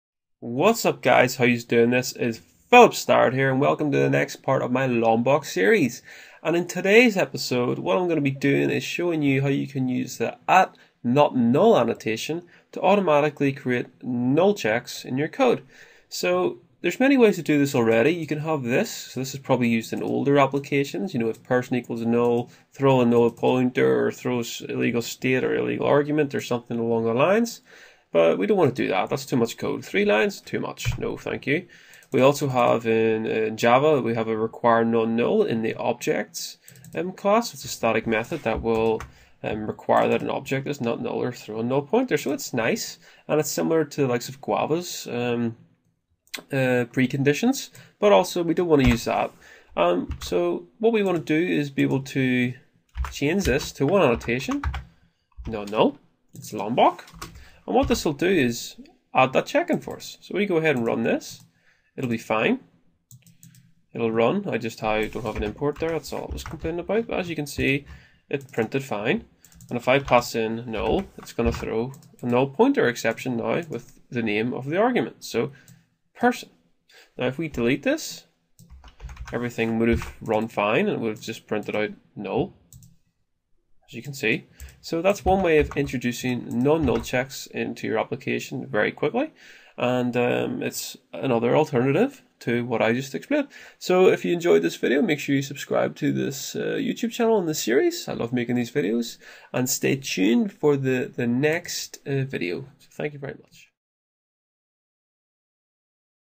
Project Lombok Java NonNull Example (Part 9)
In this tutorial I show you how to use Project Lombok @NonNull annotation in Eclipse IDE.

What is project Lombok and why would I want it?

“Project Lombok is a java library that automatically plugs into your editor and build tools, spicing up your java.”

What this essentially means, it will automatically generate common java boilerplate code (such as getters, setters, constructors, tostring etc) with a couple of annotations. This is done at compile time and weaved into your bytecode, this means project Lombok does not need be on the executables classpath during production.

Philip